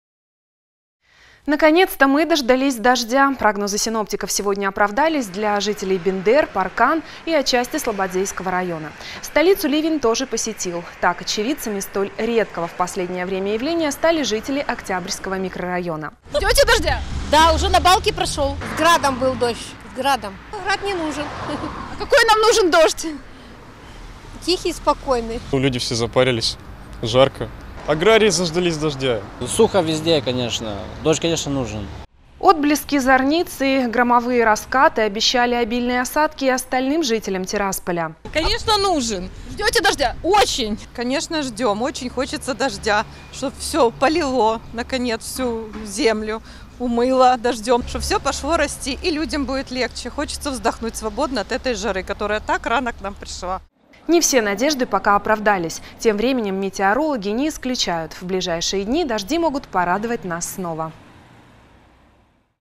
Приднестровье с важным визитом посетил дождь
Наконец мы дождались дождя! Прогнозы синоптиков сегодня оправдались для жителей Бендер, Паркан. И отчасти Слободзейского района. Столицу ливень тоже посетил. Так, очевидцами столь редкого в последнее время явления стали жители Октябрьского микрорайона.